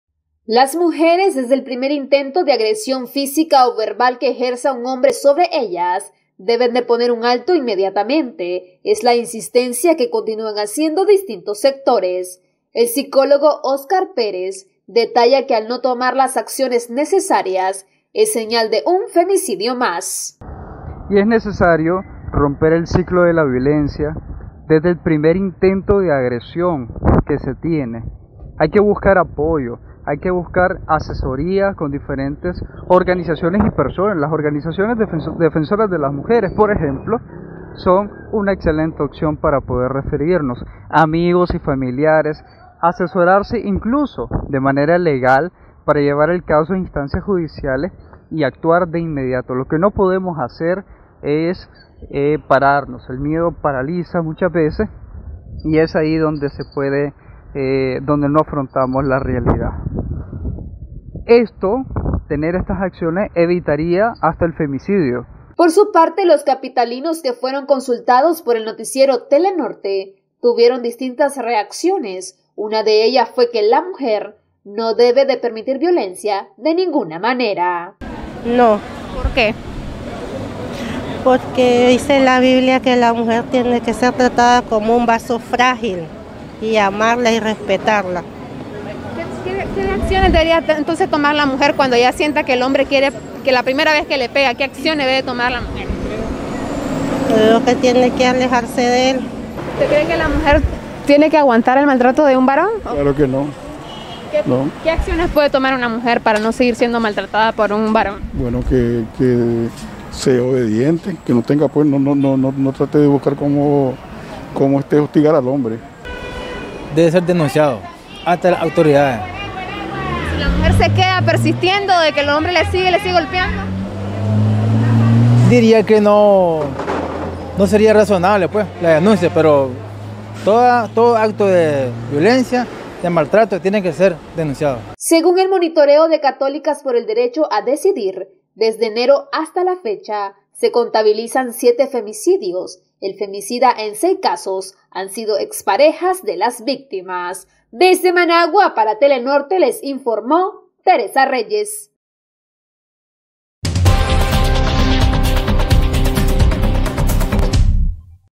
Mujer debe de tomar acciones desde el primer momento que es agredida
Distintos sectores insisten en que la mujer debe de tomar acciones desde el primer momento que es agredida física o verbalmente por su pareja para erradicar los femicidios en el país.